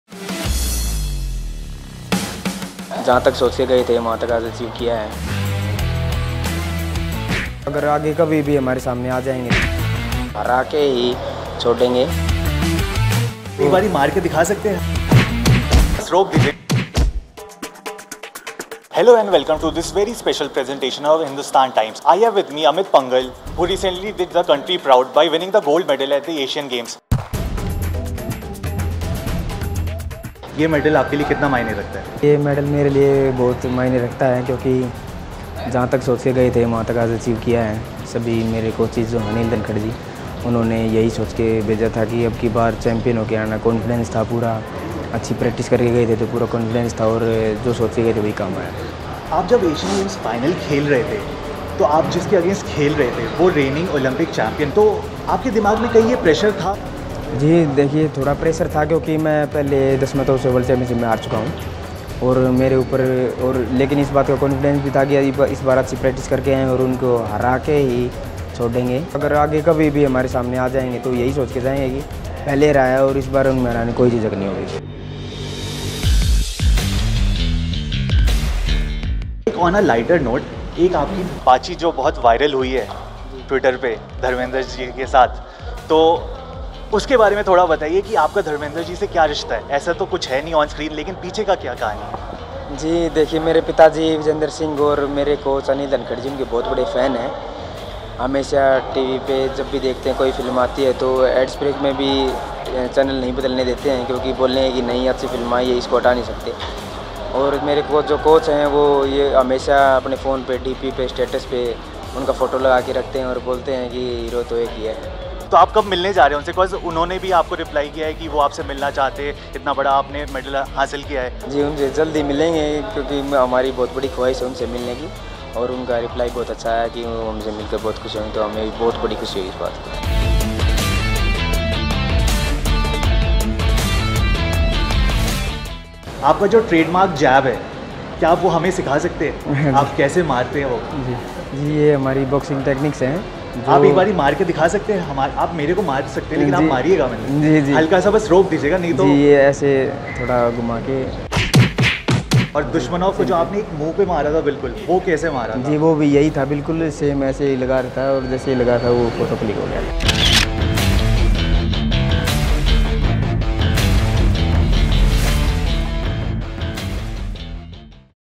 Asiad gold medallist Amit Panghal on beating Olympic champ Dusmatov, favourite move & Dharmendra
Calling his gold medal show at the recently-concluded Asian Games as no flash in the pan, ace India boxer Amit Panghal said that he is capable of beating the reigning Olympic champion Hasanboy Dusmatov again if they come face-to-face in the future. In an exclusive conversation with Hindustan Times, the boxer said that this win has boosted his confidence immensely and he is now ready to win against athletes of such caliber at bigger multi-nation tournaments like Olympics. Listen in.